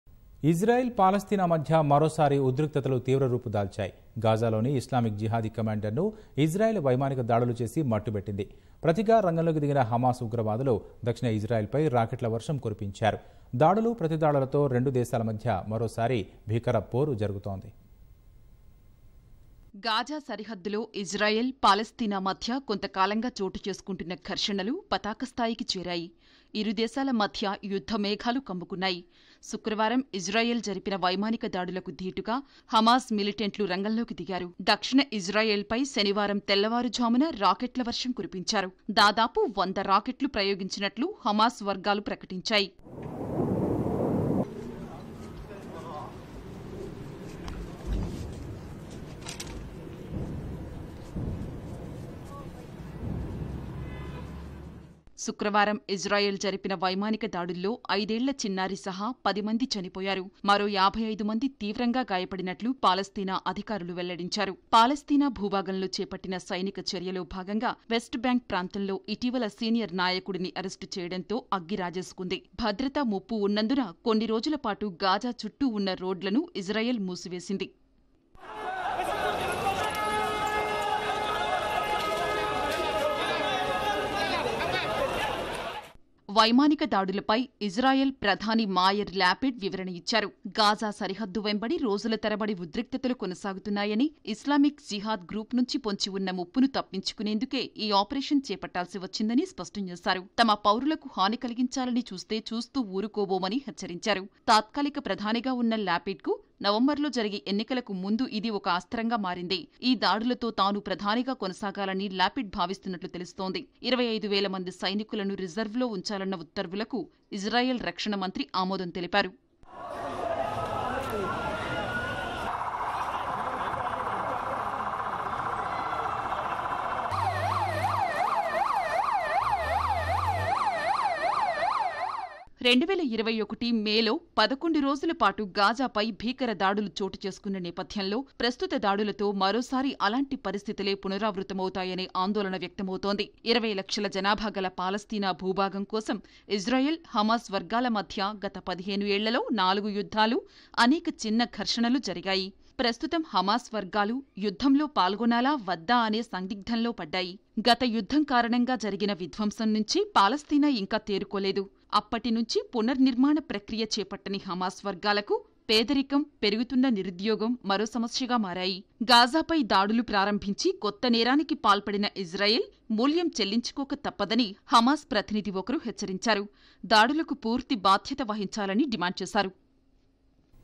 Fierce Conflict Between Israel @ Palestinian | ఇజ్రాయెల్ -పాలస్తీనా మధ్య భీకర పోరు
ఇజ్రాయెల్ -పాలస్తీనా మధ్య మరోసారి ఉద్రిక్తతలు తీవ్రరూపుదాల్చాయి. గాజాలోని ఇస్లామిక్ జిహాదీ కమాండర్ ను...ఇజ్రాయెల్...వైమానిక దాడులు చేసి మట్టుబెట్టింది. ప్రతిగా రంగంలోకి దిగిన హమాస్ ఉగ్రవాదులు...దక్షిణ ఇజ్రాయెల్ పై రాకెట్ల వర్షం కురిపించారు. దాడులు, ప్రతిదాడులతో రెండు దేశాల మధ్య మరోసారి...భీకర పోరుకు జరుగుతోంది.